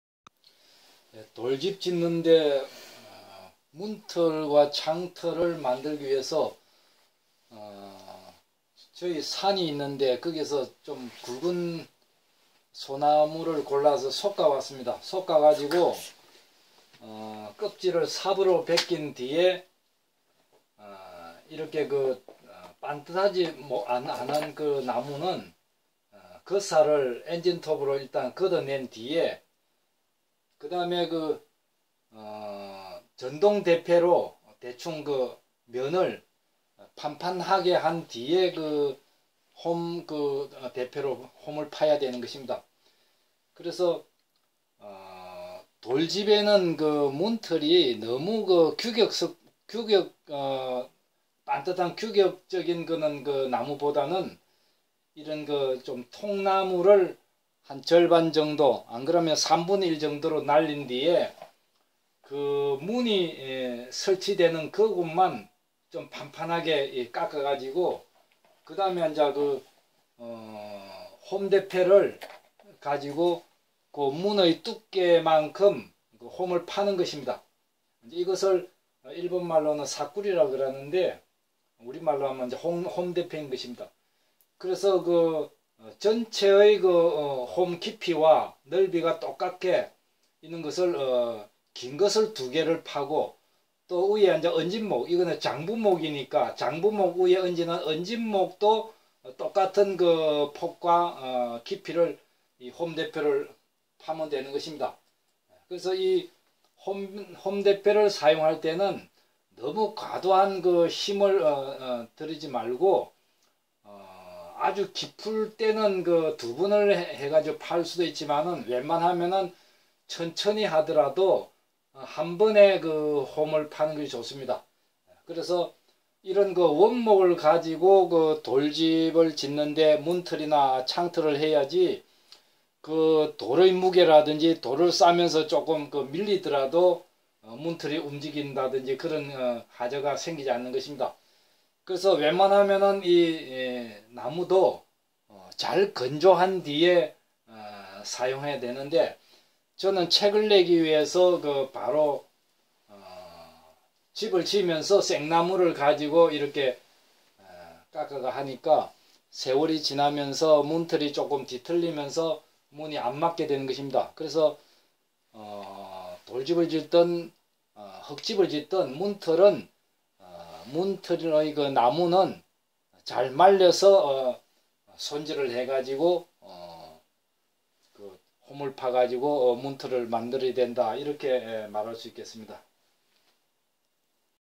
돌집 흙집학교 시공과 이론 강의 1644 / 통나무 밀어 문틀만들기
내가 지은 집
돌집짓는 방법을 연재하려 합니다.

2015년 2월 21일 무운(無耘)돌집짓기 이론 강의